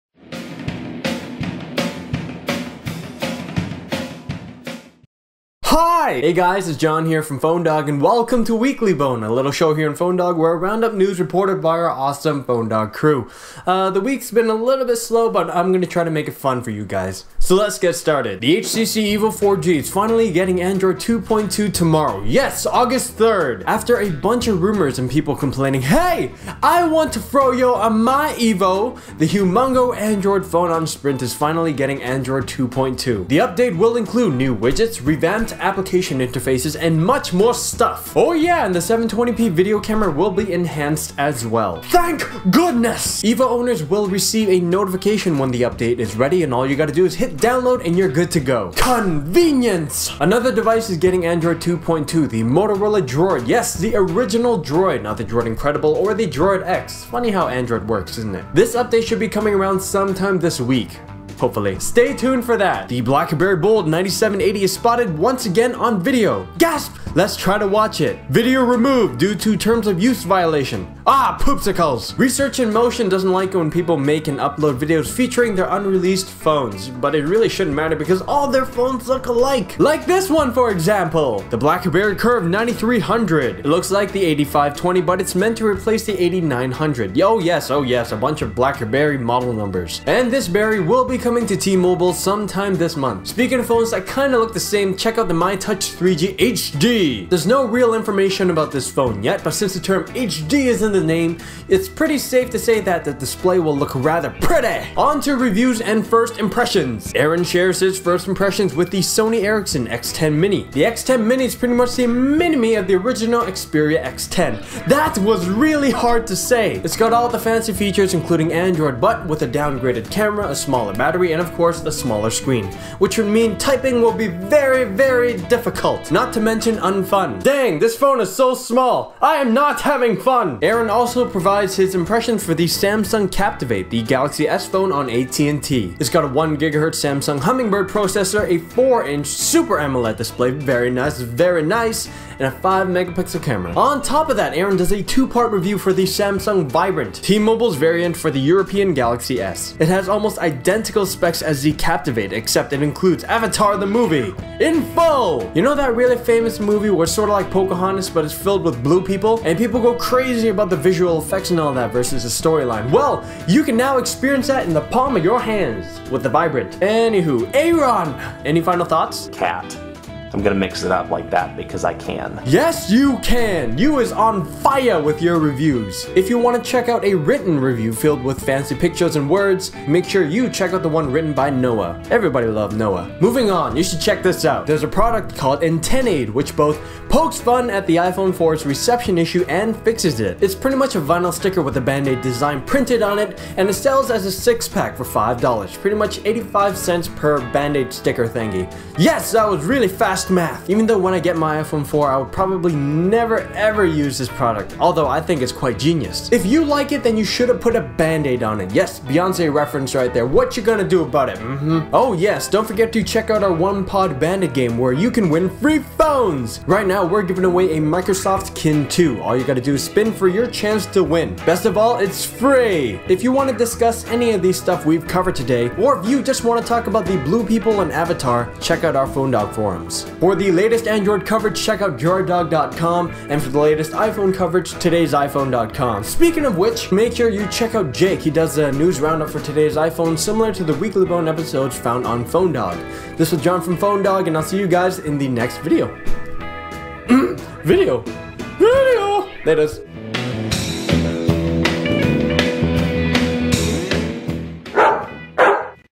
HTC Evo Gets Android 2.2 and More!
PhoneDog: Weekly Bone
Jon (TechWaffle) rounds up the latest phone news we've highlighted last week on PhoneDog in an exciting and fun video.
Win Free Phones for all the latest cell phone news, reviews, and more.